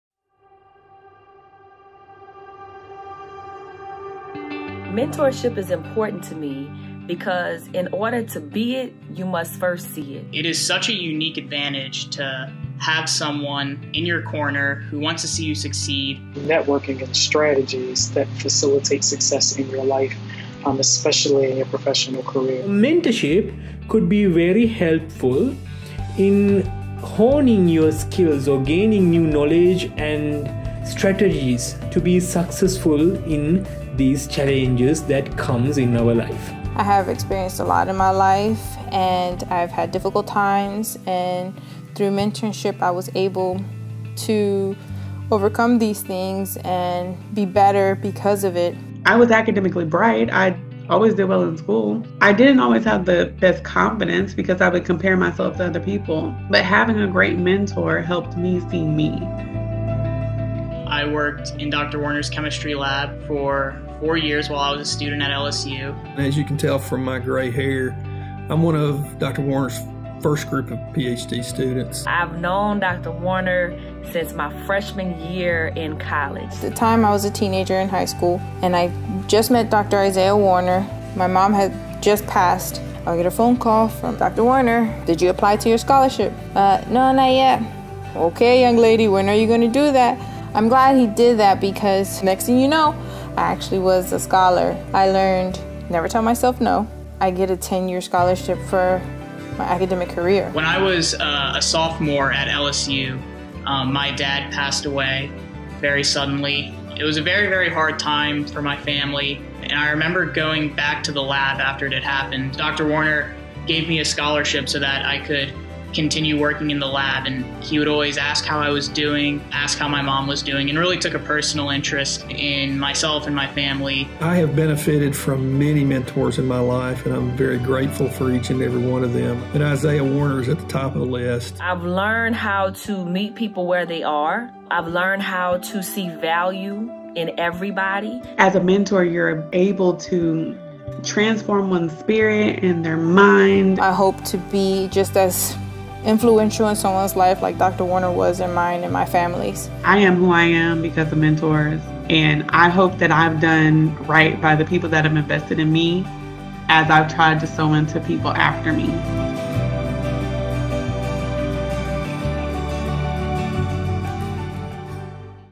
The Impact of Mentorship
Former mentees of Dr. Isiah Warner, an LSU Professor of Surface & Analytical Chemistry as well as the Vice President for LSU Strategic Initiatives, speak about the importance of mentorship and how it has impacted their lives.